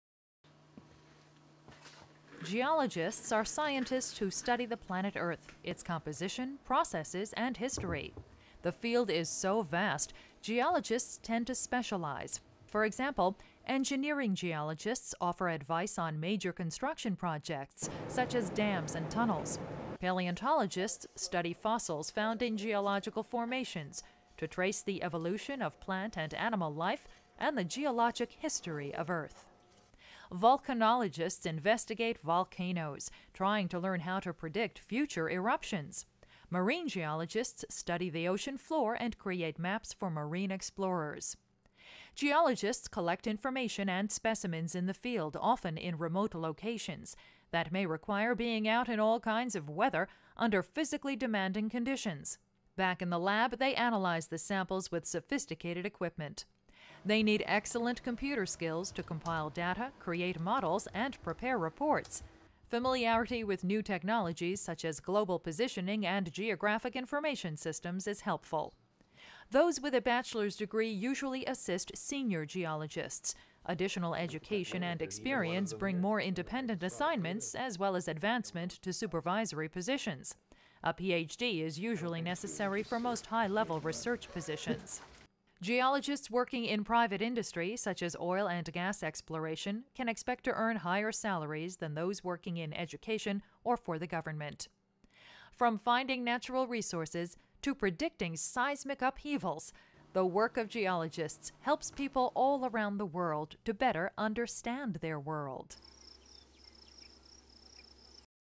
Career Planning Video: Geoscientists, Except Hydrologists & Geographers
Study the composition, structure, and other physical aspects of the earth. May use geological, physics, and mathematics knowledge in exploration for oil, gas, minerals, or underground water; or in waste disposal, land reclamation, or other environmental problems. May study the earth's internal composition, atmospheres, oceans, and its magnetic, electrical, and gravitational forces. Include mineralogists, crystallographers, paleontologists, stratigraphers, geodesists, and seismologists.